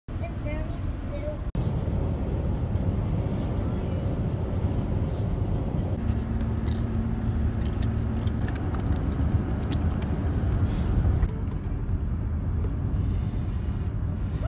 A Bernese Mountain dog enjoyed car trip
Bernese Mountain dog had a fun ride
Living his best life